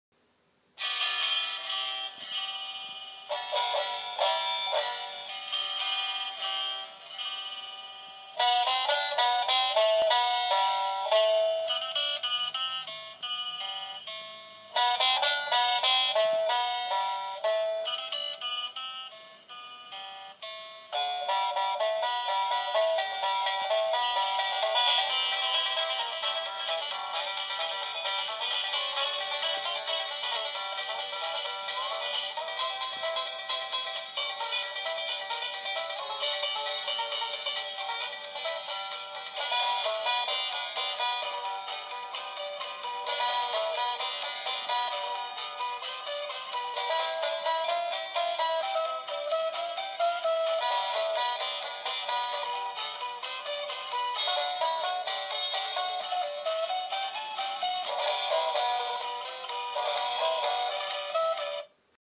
Animated dueling banjo bears - "Red and Buck"
These are my interactive dueling banjo bears made by Gemmy i'm still looking for the dancing hamsters buck and chuck.